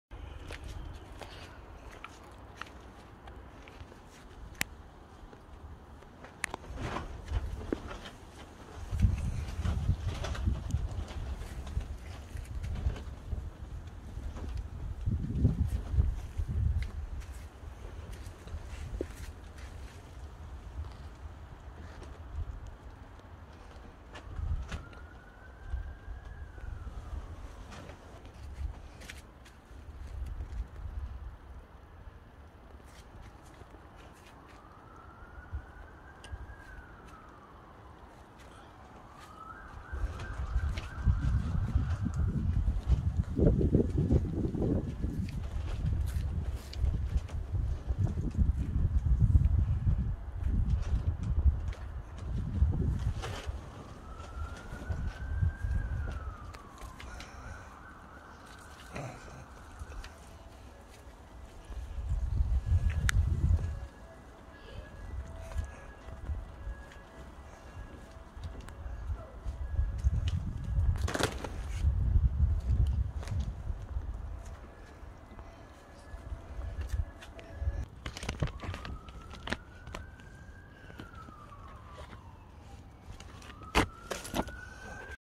winter damage but why?how to look after your garden?in the winter time#viral#desi#U.S.A#UK#subscribe
winter damage but why? how to look after your harden in the winter time in uk